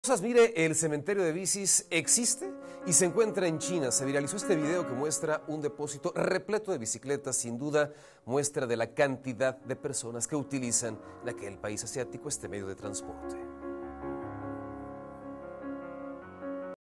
¡El cementerio de bicis existe! | Imagen GDL con Ricardo Camarena
En China existe un depósito enorme de bicis.